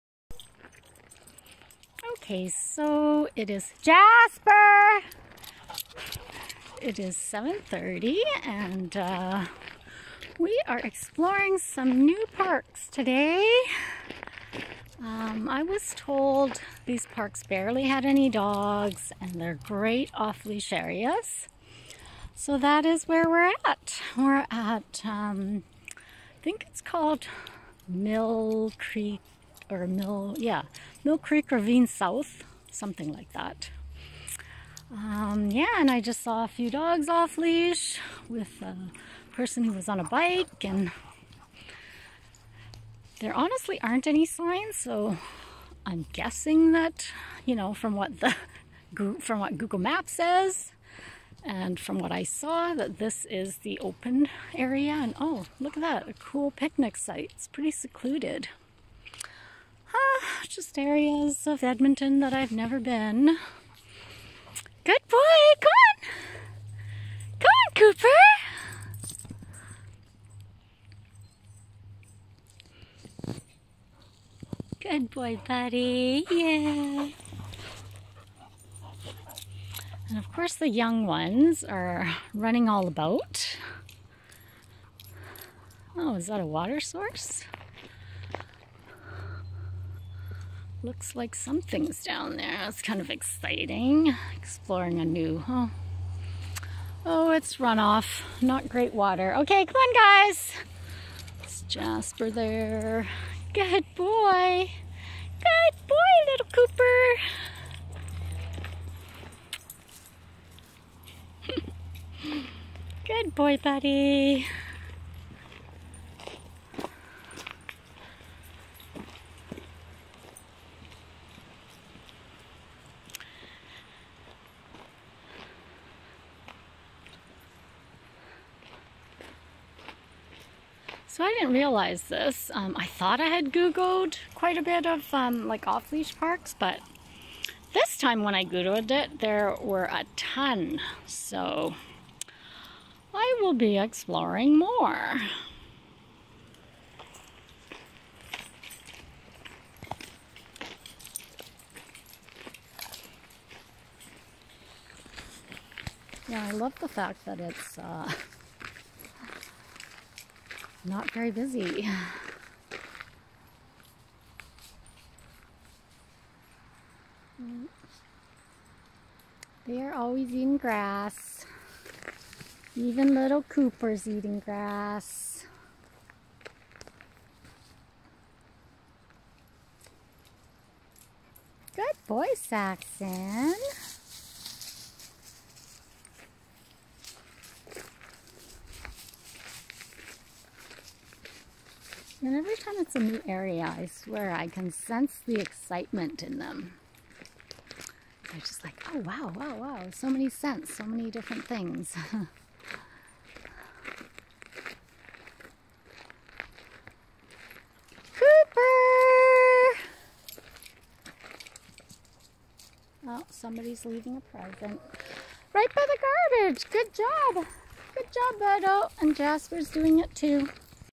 At Millcreek Ravine with Poncho, Murray, Cooper, Saxon and Jasper.(4)
The trail is great but ran into a couple of Coyotes. Going to avoid going on trails in the late evening, although the previous encounter was in the middle of the day 😳. They are getting more brazen or desperate or both and don't seem to have a fear of humans anymore. 😳😳